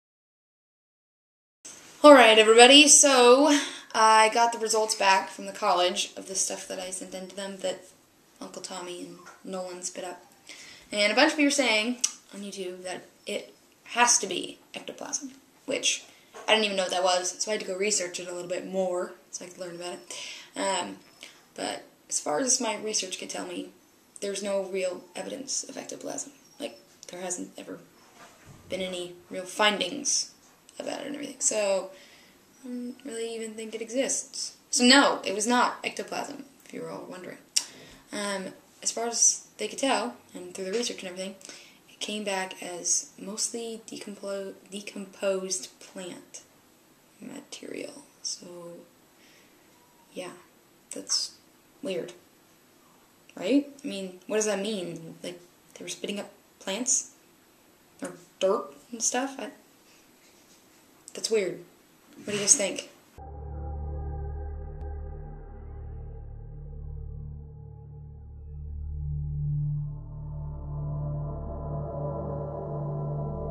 Real Haunted Houses - Ghost Ectoplasm Forensics - Results!
So I got the results back from the college.  If you remember I said I sent some of the stuff Uncle Tommy and Nolan spit up that one super creepy night into a local college to have them analyze the ghost forensics.  Well the results are interesting.  First off, no it didn't come back as ectoplasm.  They laughed when I said it might be that.  No they said it's mostly decomposed plant material.  What do you think of that?

Así que me devuelve los resultados de la universidad. Si te acuerdas que dije que envié algunas de las cosas y el tío Tommy Nolan escupir que una noche espeluznante estupendo en una universidad local para que ellos se analizan. Así los resultados son interesantes. En primer lugar, no, no lo hizo volver como ectoplasma. Se rieron cuando dije que podría ser eso. No se dice que es sobre todo el material descompuesto del pantalón. ¿Qué piensa usted de eso?

Alors j'ai eu les résultats du collège. Si vous vous souvenez que j'ai dit, j'ai envoyé quelques-uns des slideshows Oncle Tommy et Nolan cracher qu'une nuit ultra effrayant dans un collège local pour les faire analyser. Eh bien les résultats sont intéressants. Tout d'abord, il n'a pas pas revenir comme un ectoplasme. Ils ont ri quand j'ai dit qu'il pourrait être cela. Non ils ont dit c'est surtout décomposée matériels pantalon. Que pensez-vous de cela?

Also habe ich die Ergebnisse wieder aus dem College. Wenn Sie daran denken, sagte ich schickte ich einige der Sachen Onkel Tommy und Nolan spucken, dass eine Super gruselige Nacht in ein lokales College habe sie zu analysieren. Nun, die Ergebnisse sind interessant. First off, nein, es kam nicht wieder, wie Ektoplasma kommen. Sie lachte, als ich sagte, es könnte so sein. Nein, sie sagte es meistens Hose Material zerlegt. Was halten Sie davon?

だから私は戻って大学からの結果を得た。あなたが覚えていれば、私はものアンクルトミーの一部を送信し、ノーランは彼らがそれを分析しているために地元の大学にその1超不気味な夜を吐き出すと述べた。さて結果は興味深いものです。まず、全くそれはエクトプラズムとして戻ってきませんでした。私はそれがいるかもしれないと言ったとき、彼らは笑った。いいえ、彼らはそれが主に植物材料を分解して言いませんでした。あなたはそのことをどう思いますか？

Additional information:
Paranormal activity
Ghosts caught on tape
Scary ghosts
Paranormal